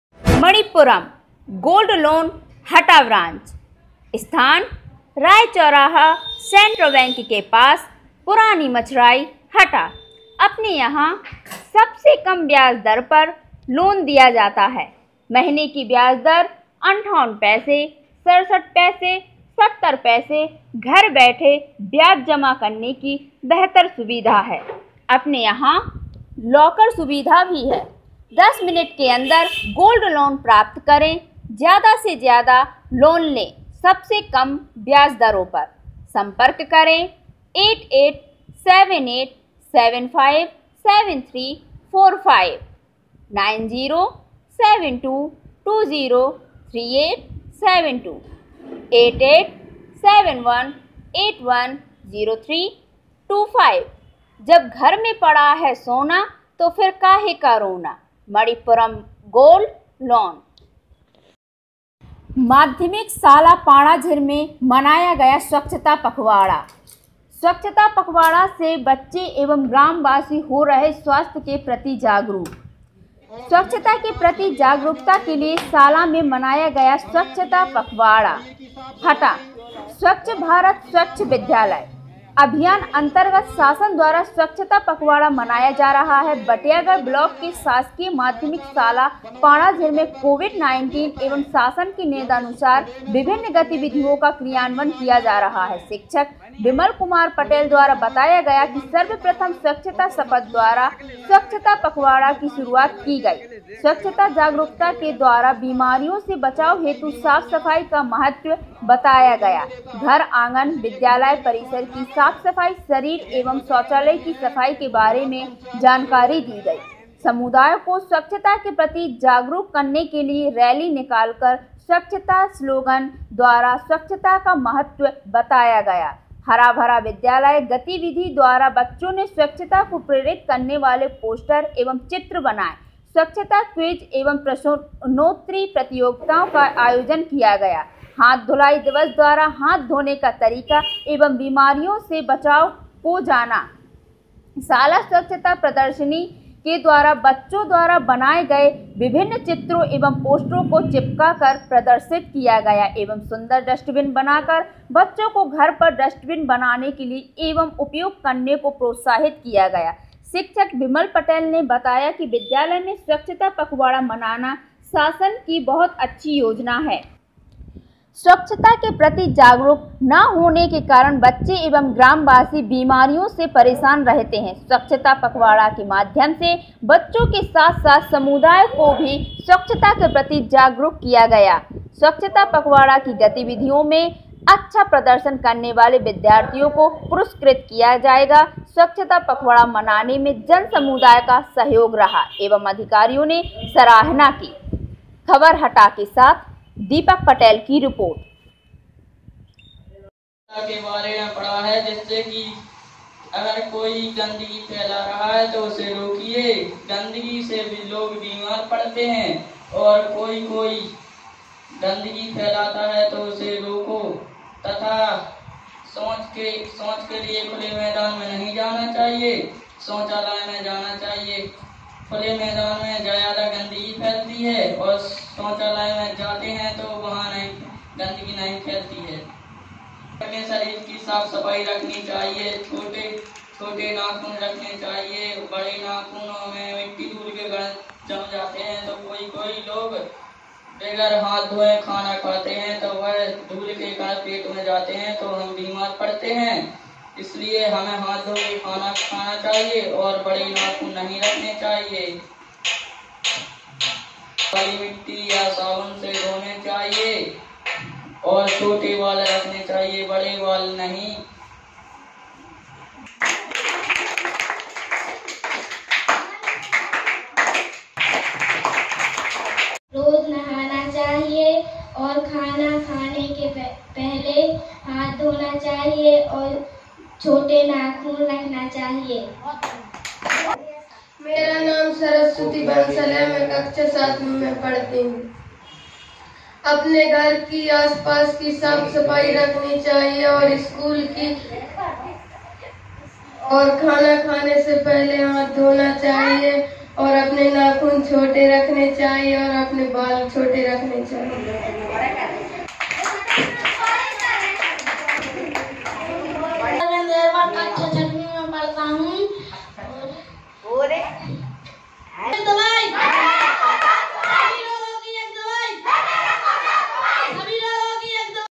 विमल कुमार पटेल द्वारा बताया गया कि सर्वप्रथम स्वच्छता शपथ द्वारा स्वच्छता पखवाड़ा की शुरुआत की गई।
खबर हटा की